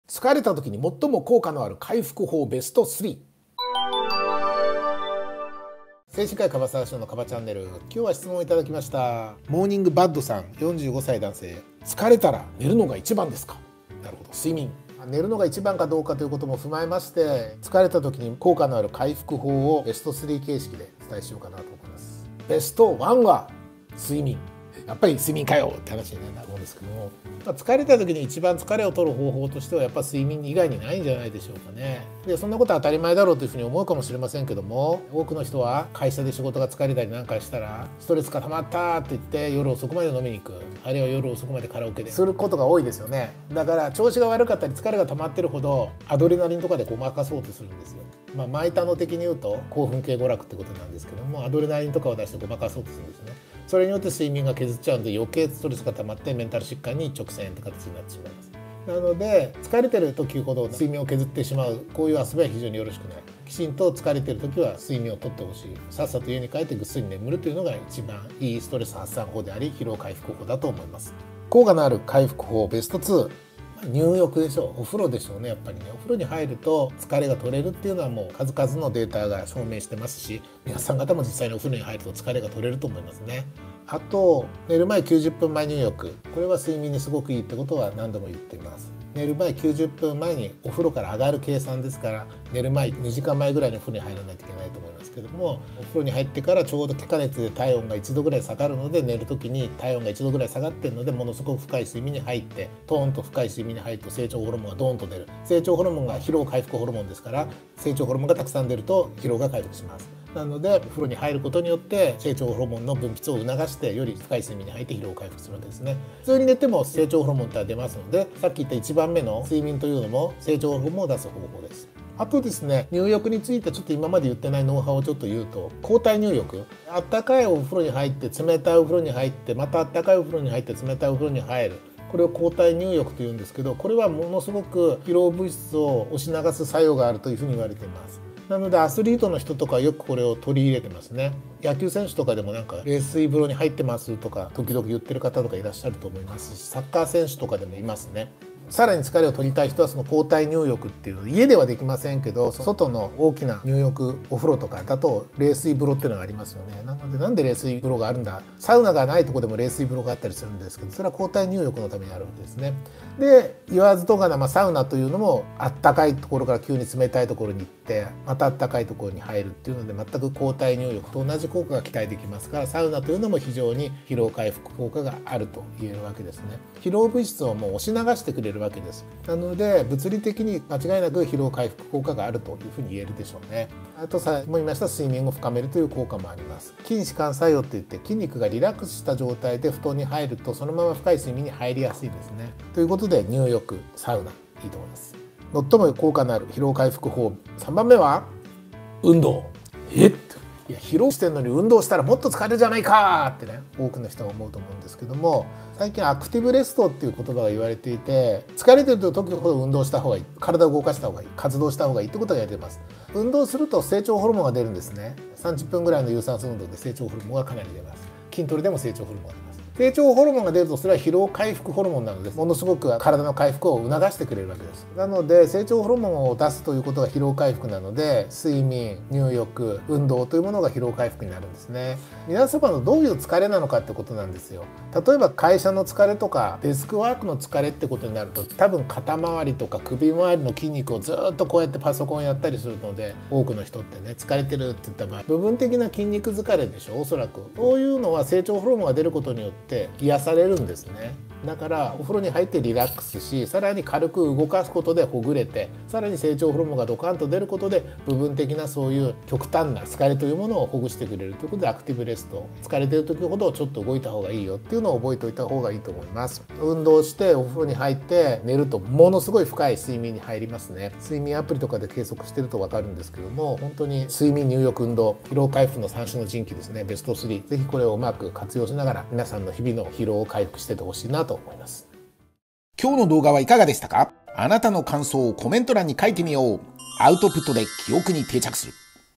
最も効果のある回復法ベスト３【精神科医・樺沢紫苑】
【質問】「疲れたら寝るのが一番ですか？」
モーニングバッドさん（45歳・男性）